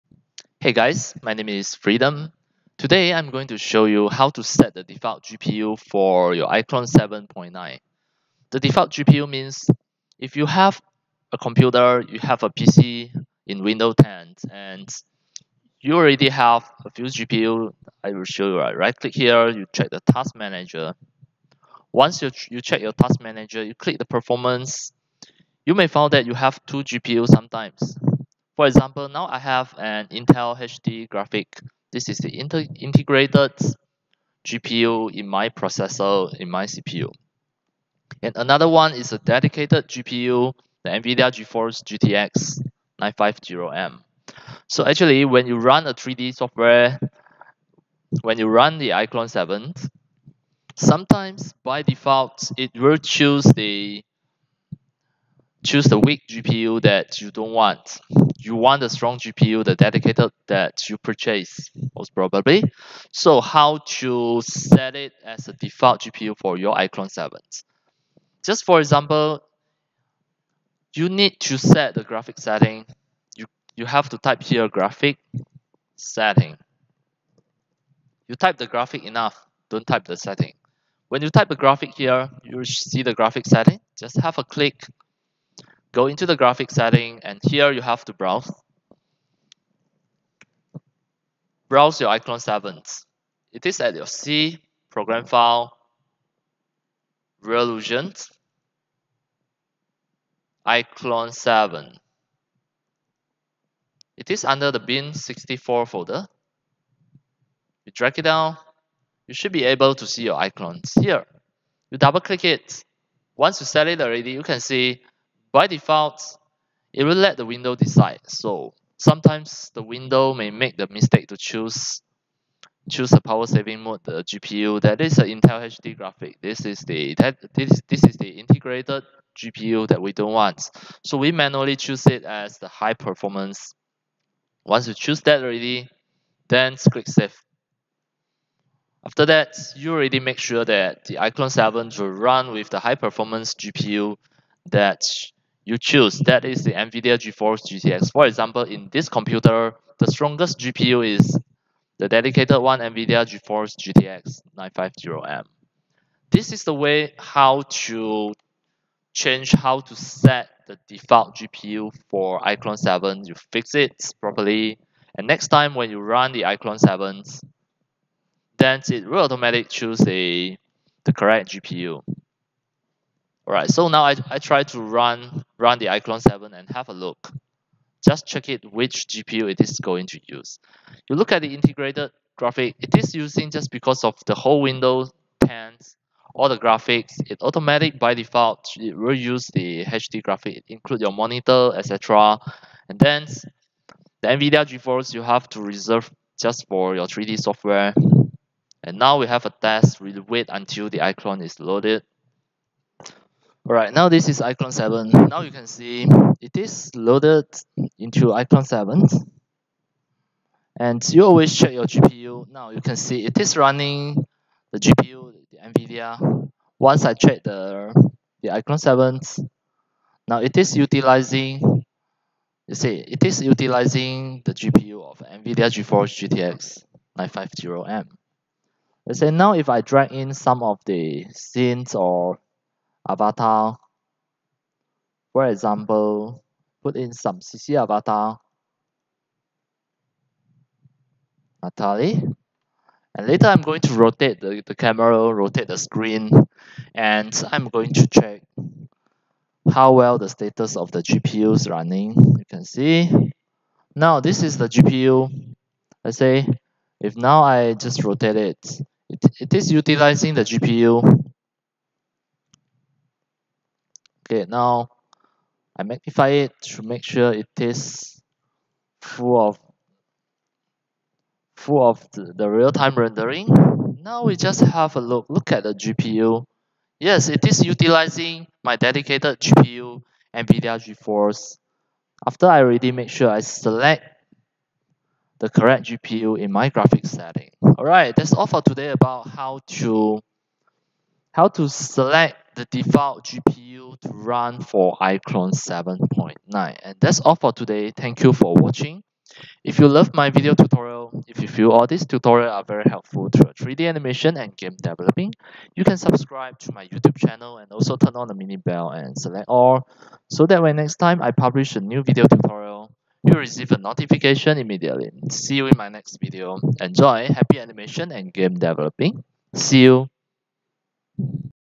How to set default GPU for iClone 7.9 if you have two? Full Tutorial -
- Hey guys, how to set the default GPU for your iClone 7.9 if you have two GPU in your PC? How to makesure the iClone 7.9 really utilize the GPU that you want to utilize? You can manually select the GPU that you want your iClone 7.9 to utilize. This is my step-by-step tutorial on how to do this! ENJOY! and have fun! Happy animation and game dev! ENJOY!

Get iClone and CC3 Contents

Your friend,
FREEDOM